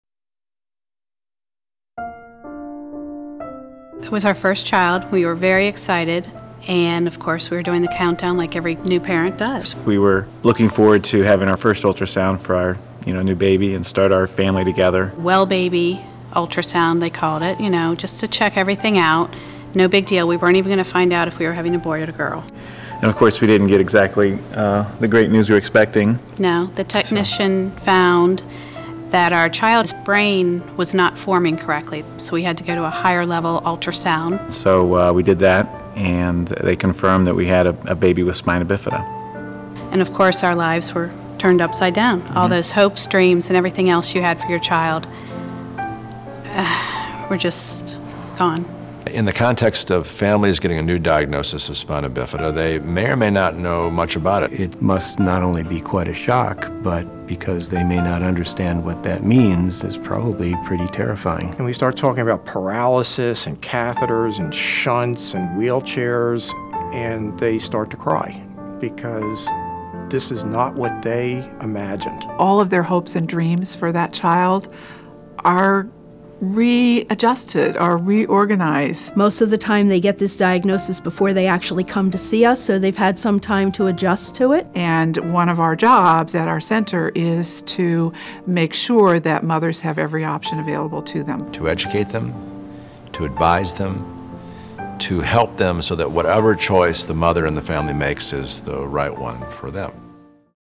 Birth of a Breakthrough: Treating Spina Bifida Before Birth (1 of 12)
For babies prenatally diagnosed with spina bifida, Children’s Hospital of Philadelphia offers the most advanced treatment options.

Decades of work at Children’s Hospital of Philadelphia’s (CHOP) Center for Fetal Diagnosis and Treatment has helped to develop a remarkable breakthrough surgery option to treat spina bifida before birth and lessen its lifelong impact. In this video, you’ll hear from a patient family who shares how they felt when they received a spina bifida diagnosis, and meet some of the team of fetal surgery experts at Children’s Hospital of Philadelphia.